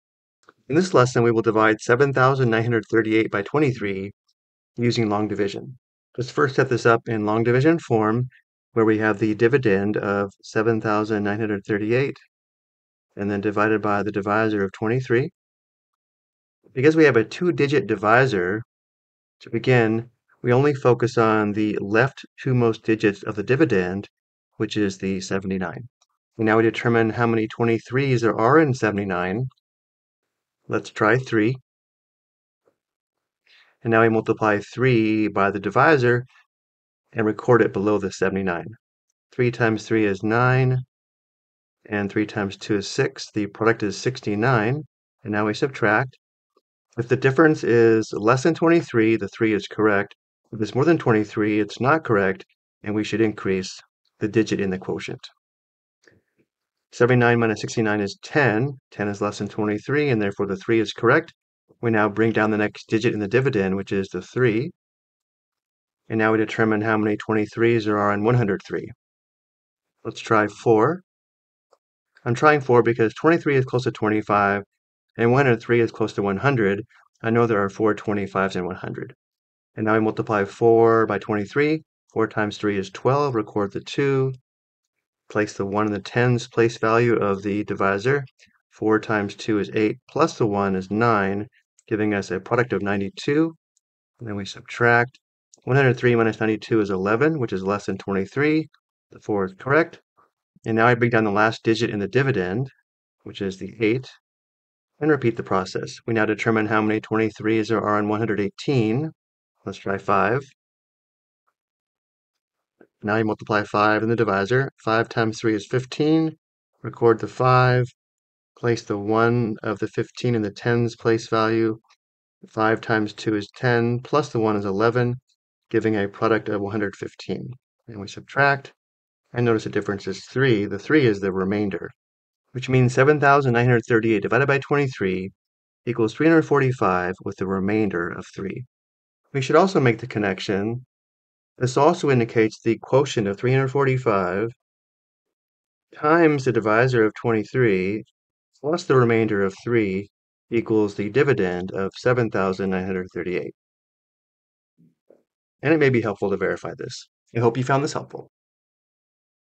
Long Division: 4 Digit Divided by a 2 Digit - With Remainder
This video explains how to perform long divisino using the traditional algorithm.